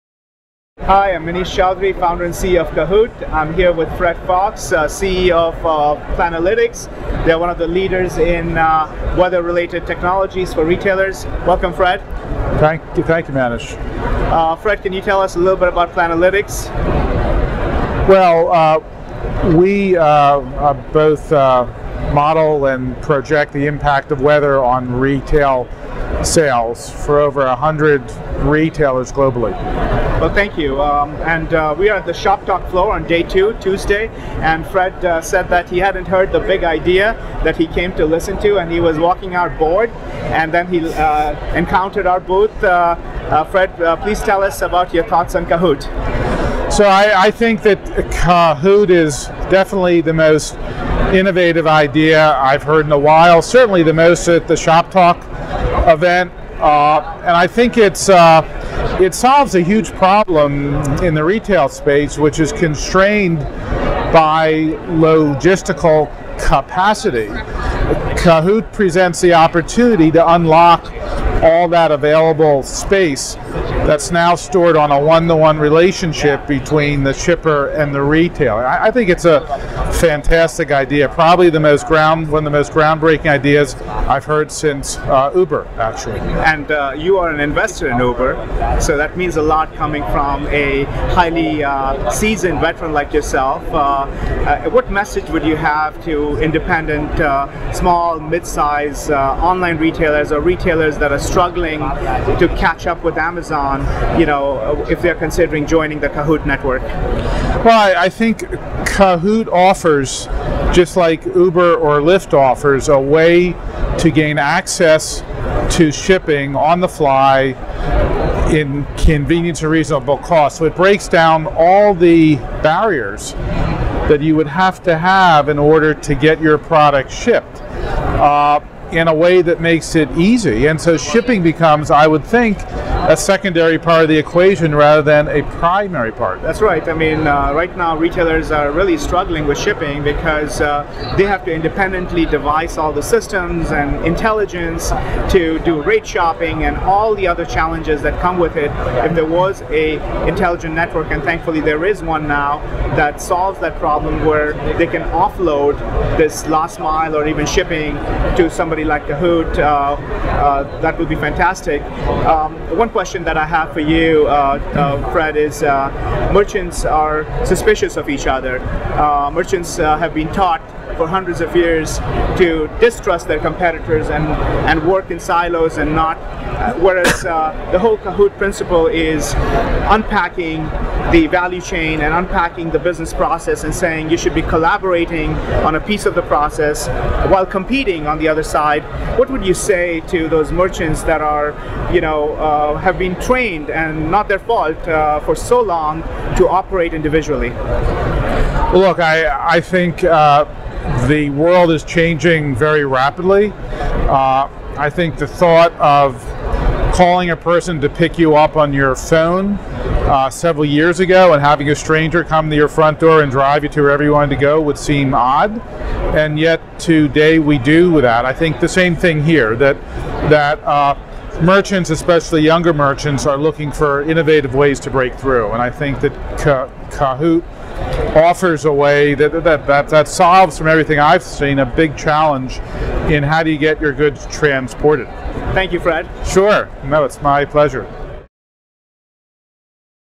Order Fulfillment Disruptive Ecommerce - Cahoot & Planalytics at ShopTalk 2019
Cahoot is a revolutionary concept in ecommerce order fulfillment. Offer your customers 1-day and 2-day shipping at ground rates or less.

You can offer nationwide Seller Fulfilled Prime - SFP - without blowing your budget. Our peer-to-peer network of top-rated sellers fulfill your orders nationwide without the high cost of multiple warehouses.